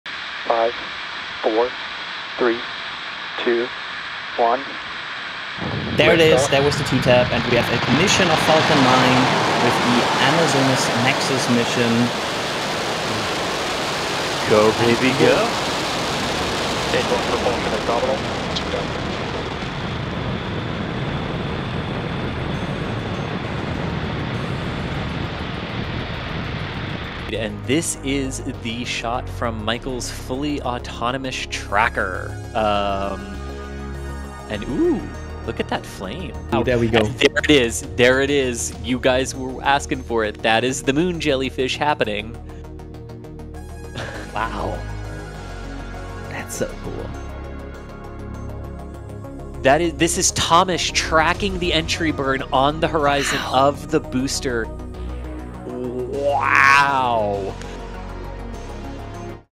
SpaceX Amazonas Nexus Mission (2/6/2023)
SpaceX launched a Falcon 9 carrying the Amazonas Nexus satellite for Hispasat. The launch from CCSFS SLC-40 headed to geostationary transfer orbit. The first stage is B1073-6 which successfully landed on Just Read The Instructions.

🔍 All content copyright to NSF. Not to be used elsewhere without explicit permission from NSF.